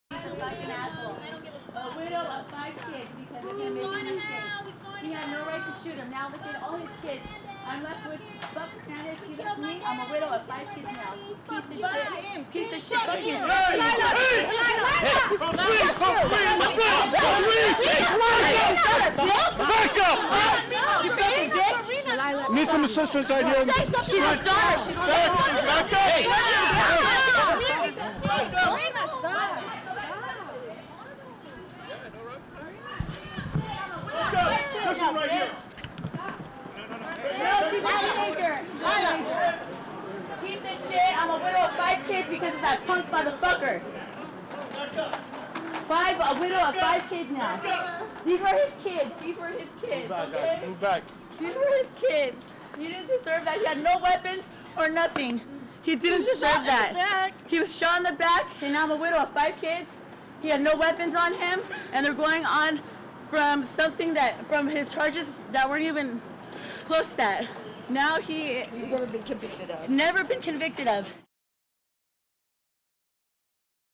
A Fight Breaks Out Between Two Families Outside San Jose Courthouse 1-18-13 (Uncensored)
Community members who support a San Jose man who killed a suspected burglar at an apartment complex  on New Year's Eve rallied Friday afternoon at the Hall of Justice to decry the murder charges filed against a "hard working soccer dad."

And just after the brief court appearance, supporters of the shooter - Luis Ricardo Hernandez - and family members of the slain man - Chris Soriano - got into a fight outside the courthouse.

Soriano's widow, Carmen Soriano, could be heard yelling, "You killed my husband!" And she talked about how her five children have no father. She also said the family is now homeless.

Fists flew, and Santa Clara County sheriff's deputies ran out to separate the warring families. Two women from each side were said to have gotten physical with each other.

No one was arrested, but deputies let the Hernandez family leave first and then the Soriano family leave.